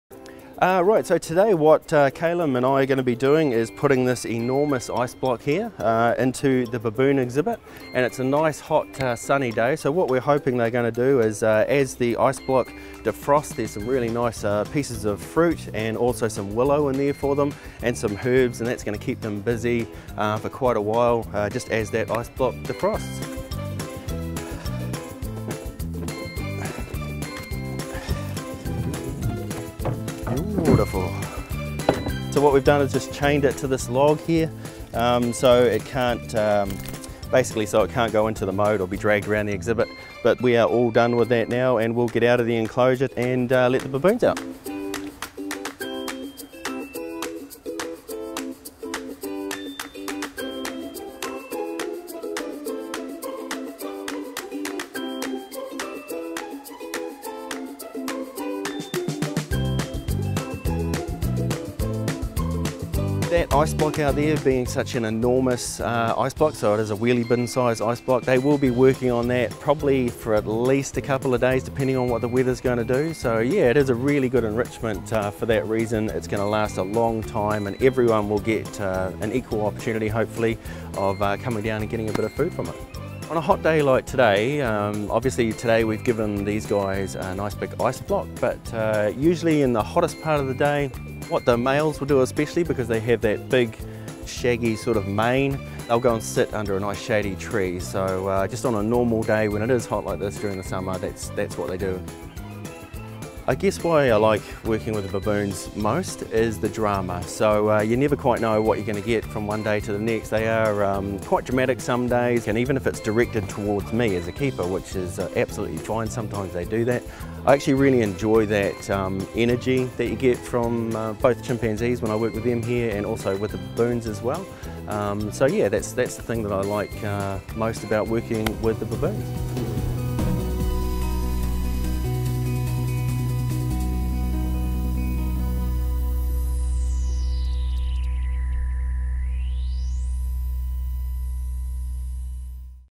Zoo Tales - Ice blocks for baboons
On a hot summer's day, an enormous ice block is the perfect treat for our troop of baboons!